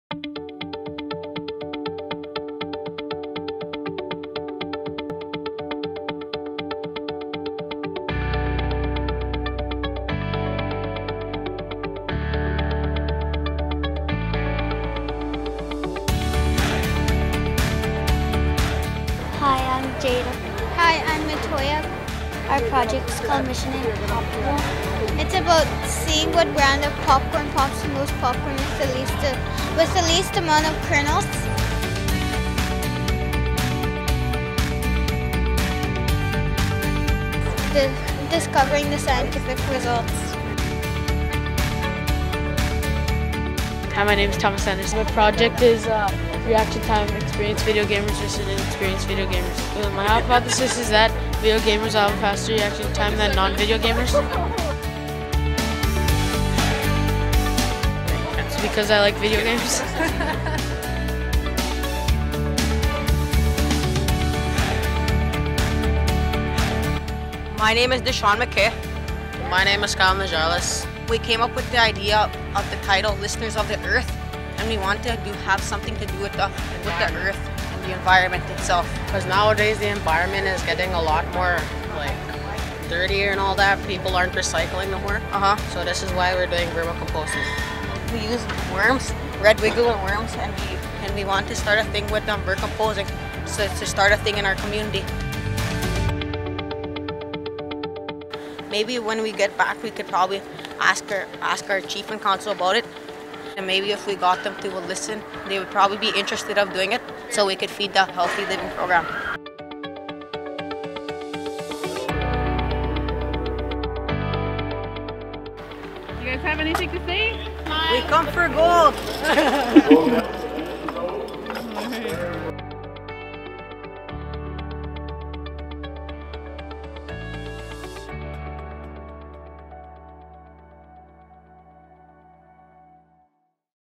Manitoba First Nations Science Fair 2017
We asked a few of the gold medal winners at the 15th annual Manitoba First Nations Science Fair to tell us about their projects. Here’s what they had to say. Congratulations to Skylin Desjarlais & Deshawn McKay of Crane River FN, Thomas Anderson, and Keaton Shorting of Little Saskatchewan FN will compete at the Canada-Wide Science Fair in Regina in May. Special congratulations to Crane River FN - each of their representatives received gold.